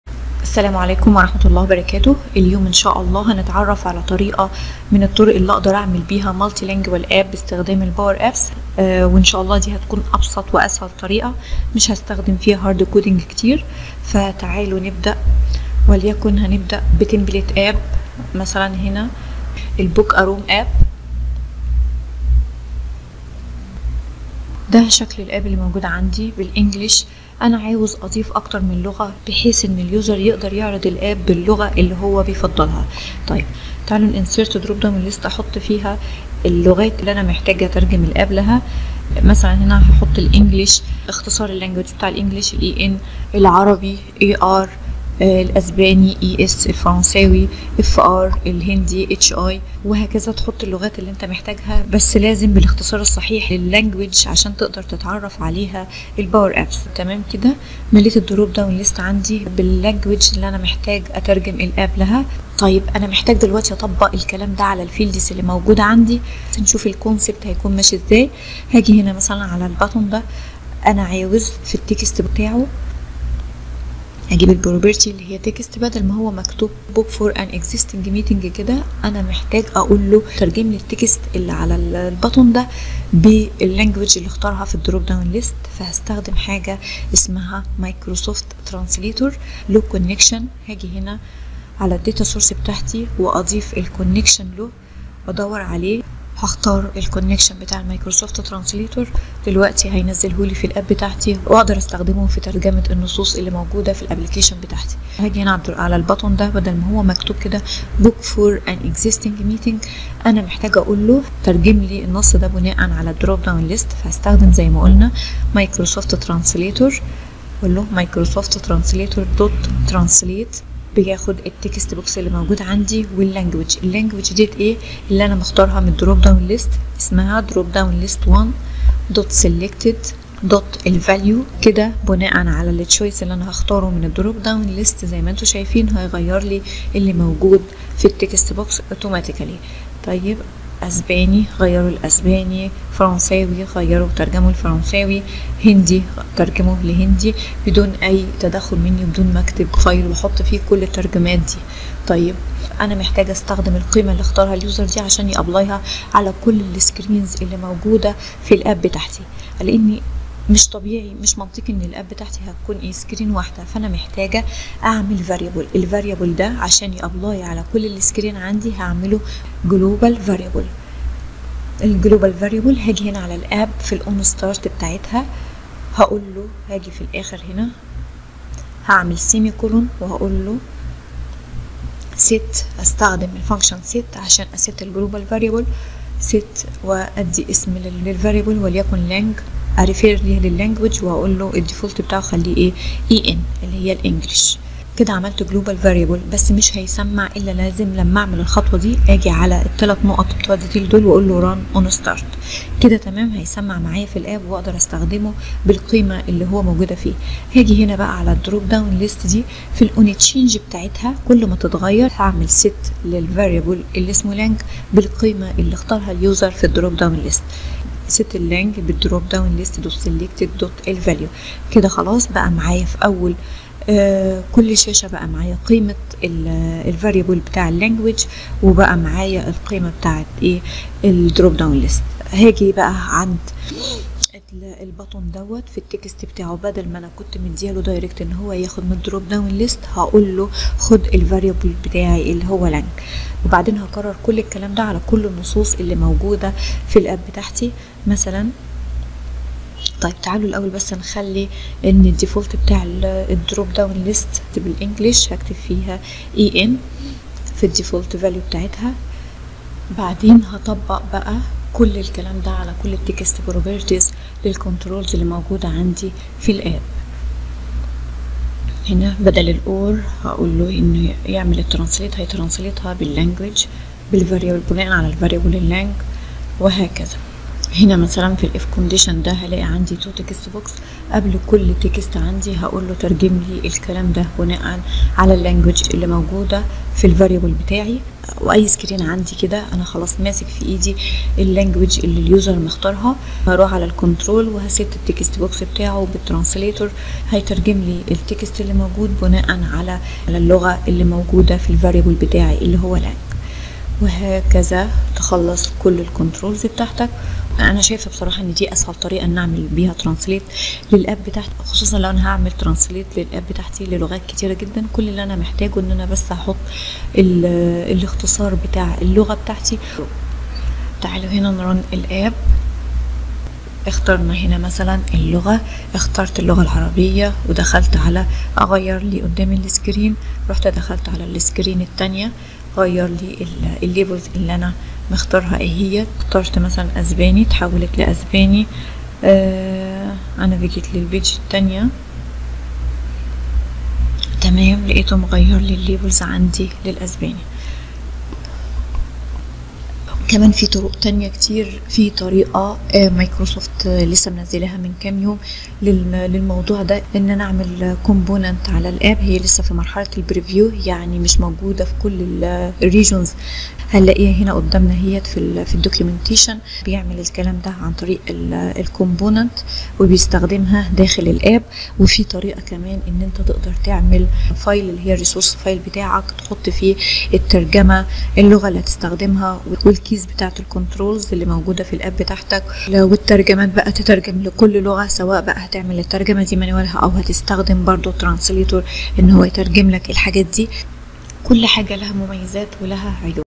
Building Multilingual Power Apps using Microsoft translator - شرح powerApps
you will learn how to build multilingual PowerApps using Microsoft translator
00:00 intro
00:15 create book a room app
00:32 Create drop down list for app languages
01:31 Add Microsoft translator connector
01:57 use Microsoft translator to translate texts
02:45 create global variable to hold the user language
03:04 set variable
03:50 Use variable to define the user language for Microsoft translator
05:55 Create translation component